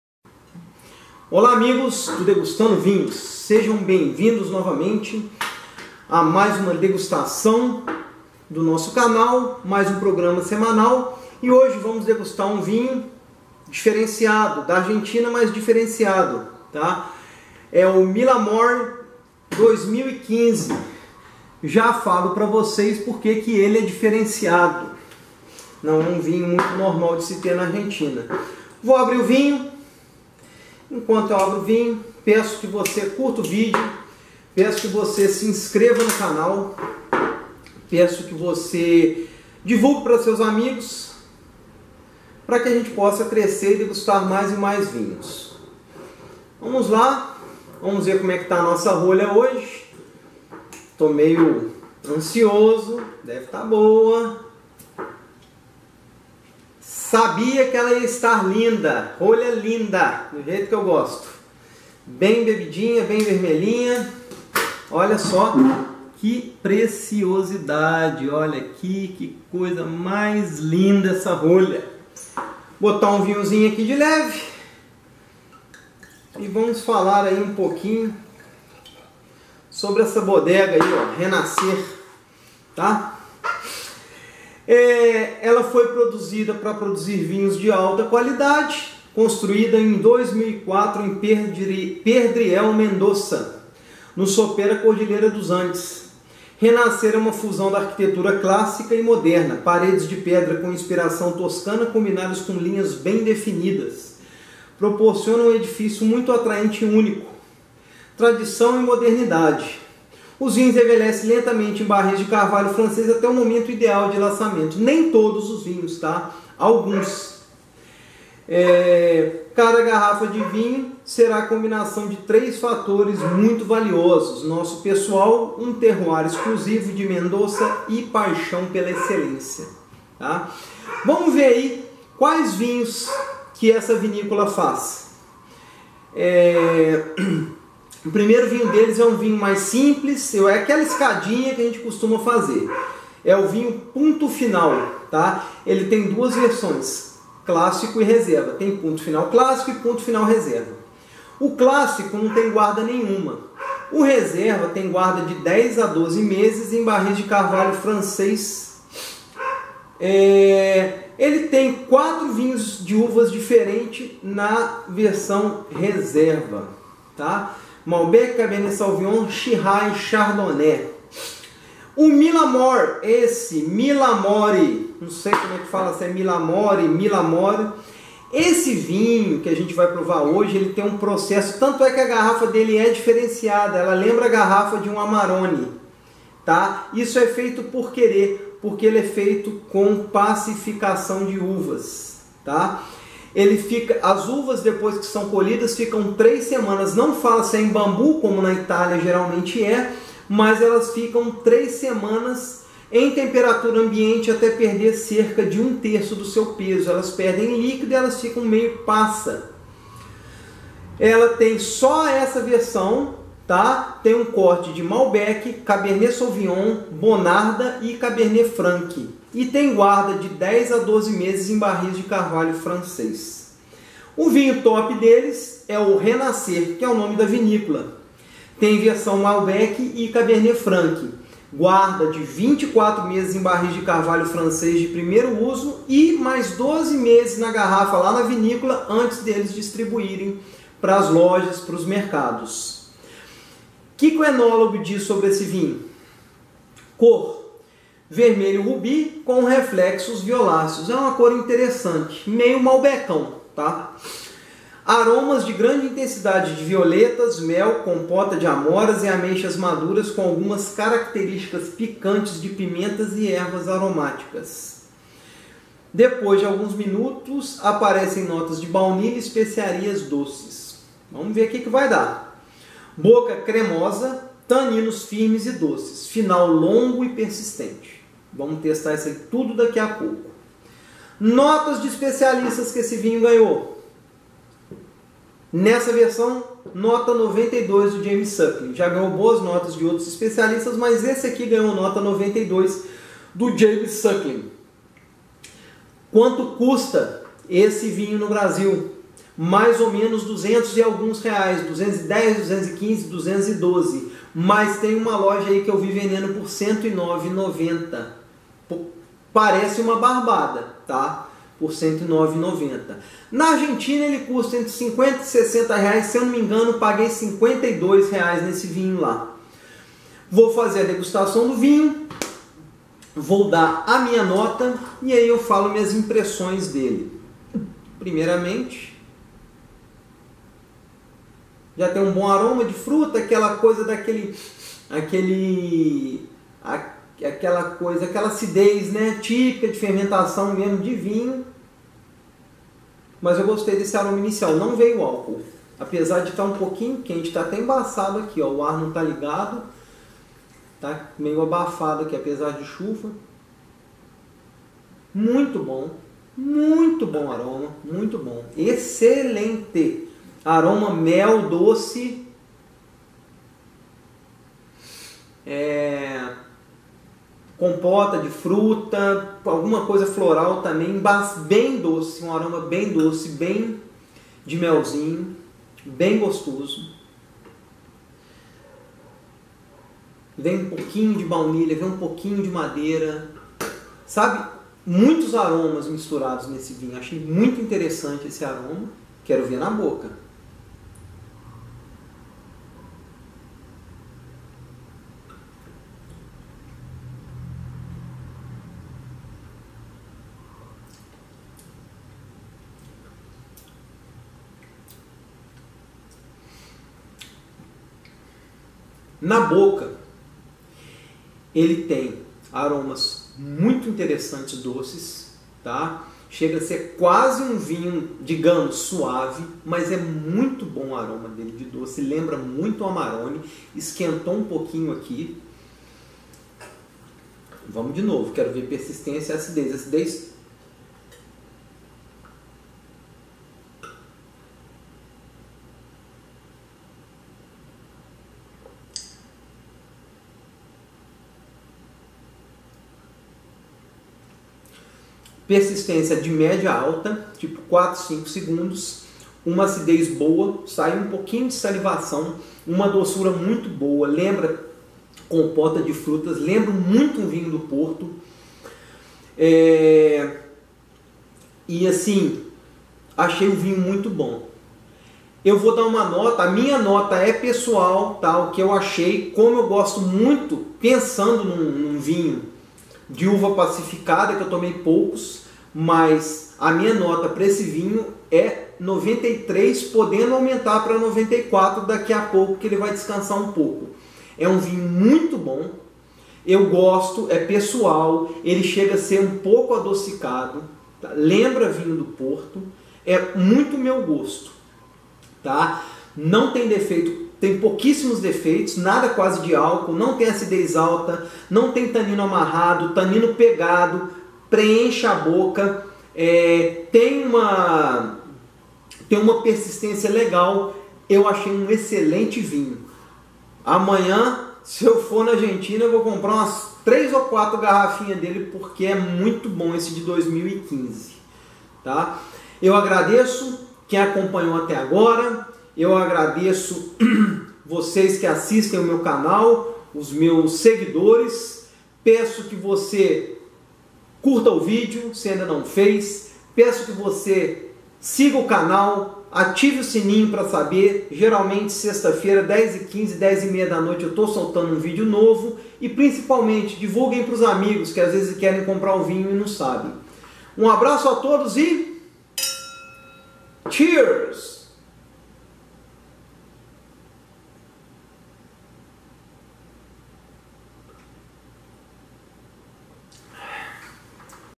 #0031 - Vinho Milamore 2015
Degustação do vinho Milamore 2015

Minha análise em: 05:26